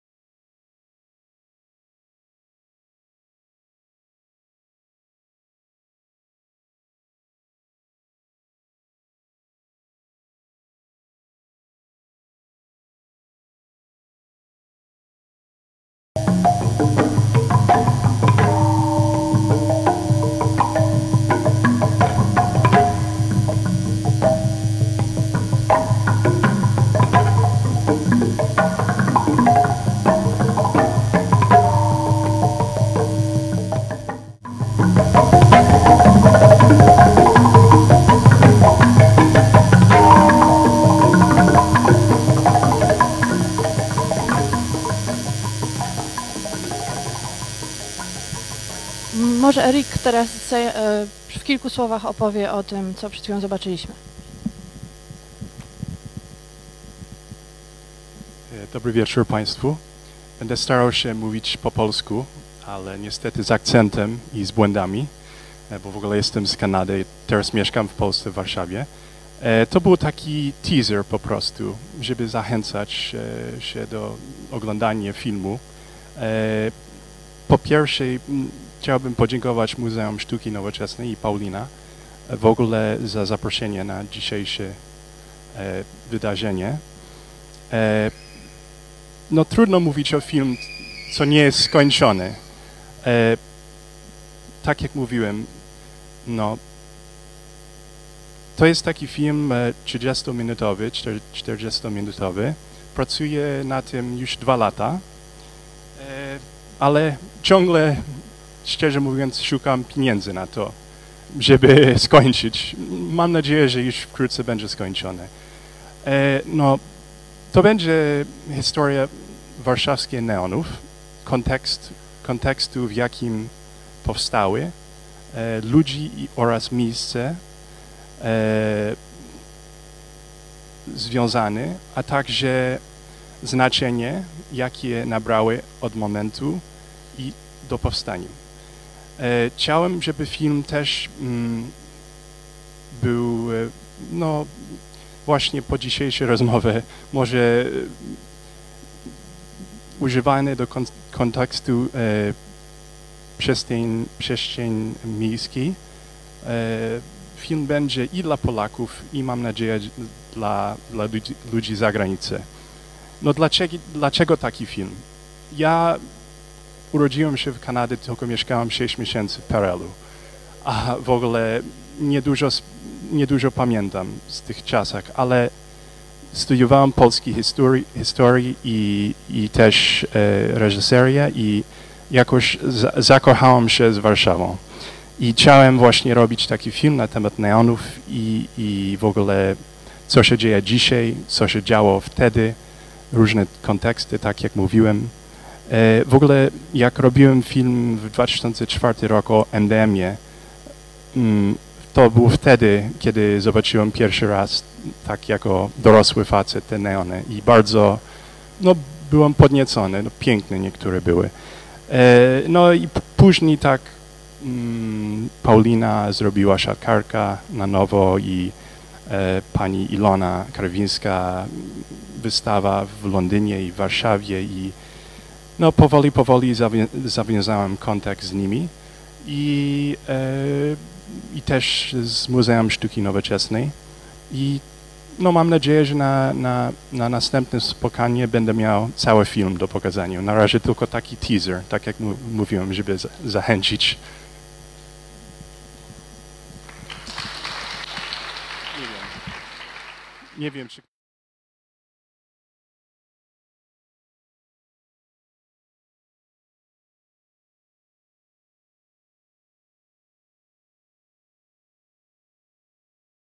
WWB TV. Życie neonów. Wystąpienie Eric'a Bednarskiego
Wystąpienie Eric'a Bednarskiego, twórcy filmu dokumentalnego „Warszawskie neony". Wystąpienie miało miejsce w ramach spotkania „Paulina Ołowska. Życie neonów".